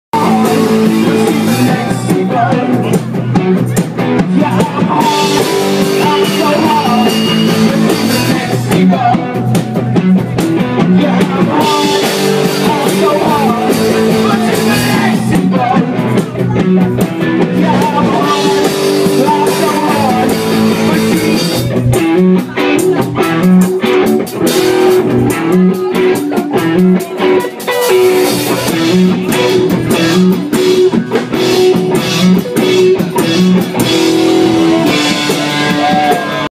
Hard Bodies at Bohemian Grove
Hard Bodies make people dance at the Bohemian Grove  3/16/13